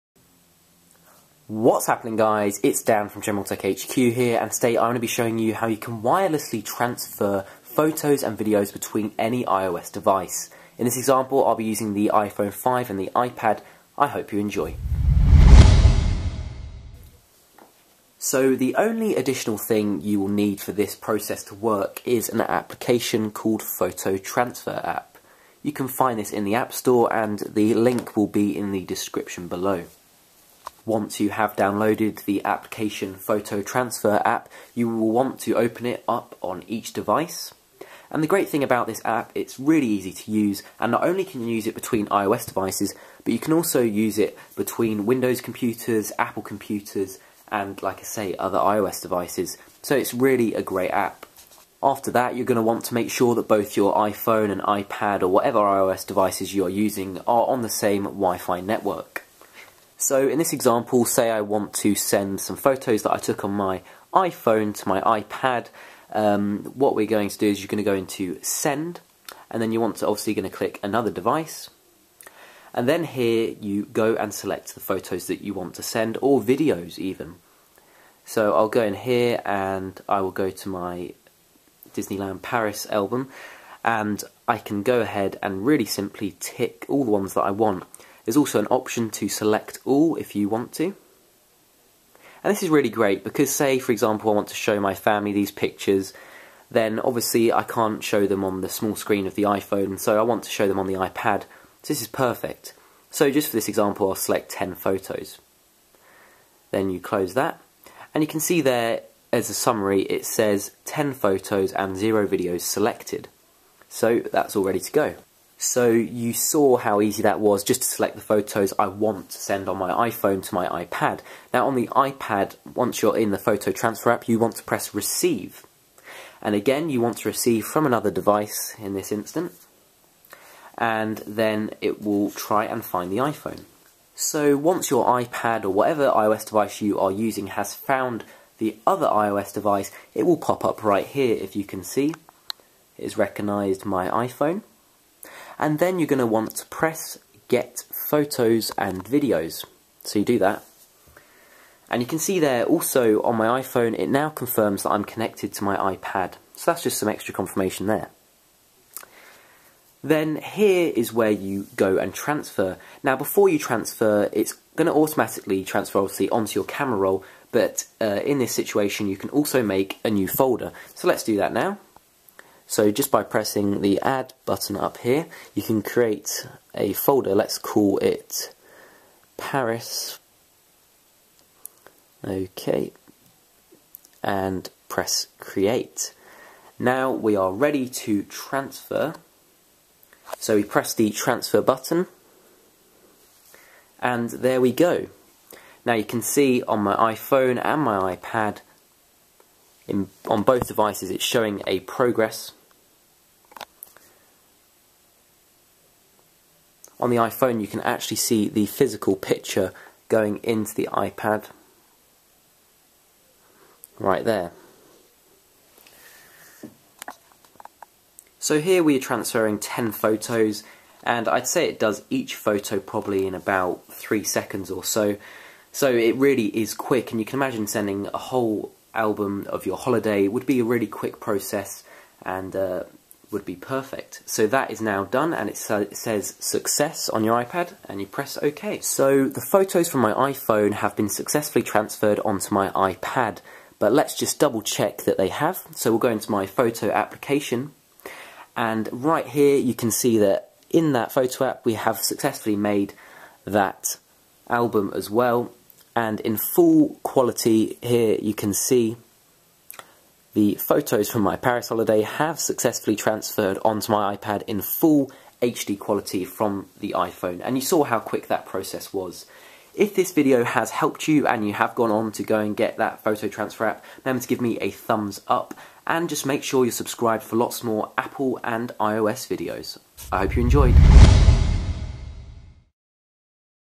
Transfer Photos & Videos Between iPhone & iPad : Easy & Wireless With PhotoTransferApp
Transfer Photos & Videos Between ANY iOS Device / Android : Easy & Wireless With PhotoTransferaApp !